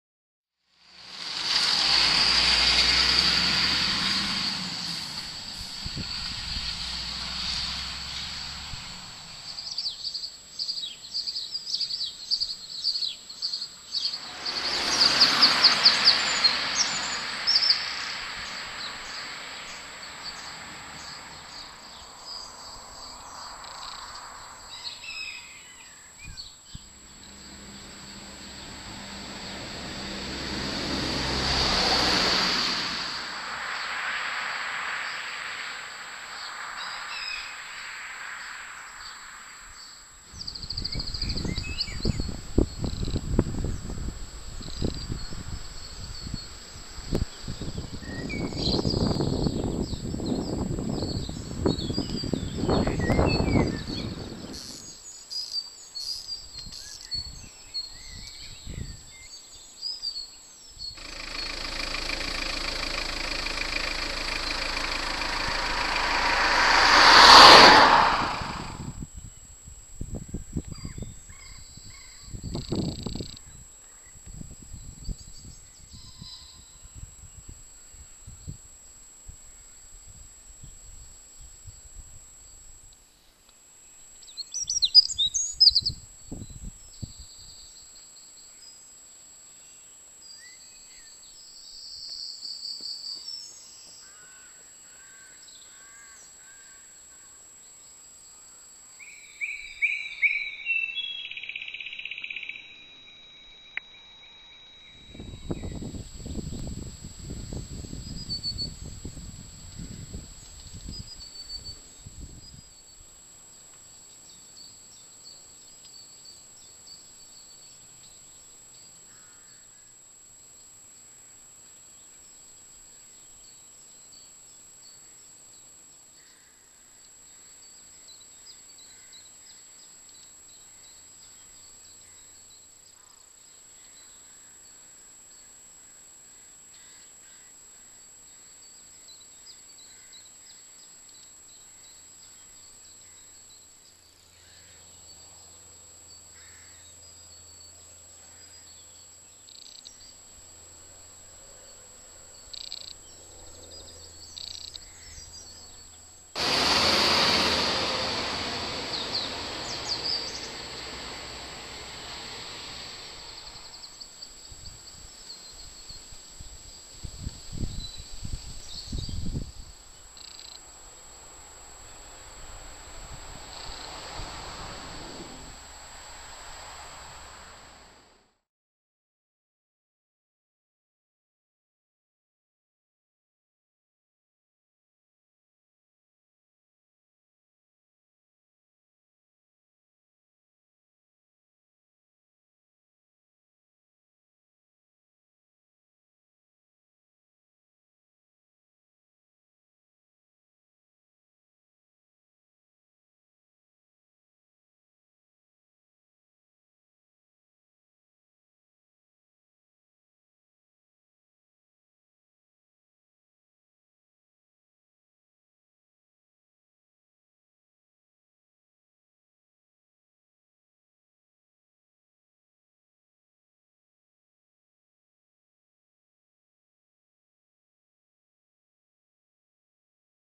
Easter Chill-Out, Video Clips on way to Kings Stag Garage and back
Filmed using Vivitar DVR 830 XHD, This was filmed on Good Friday which was nice and warm, the following Saturday is colder!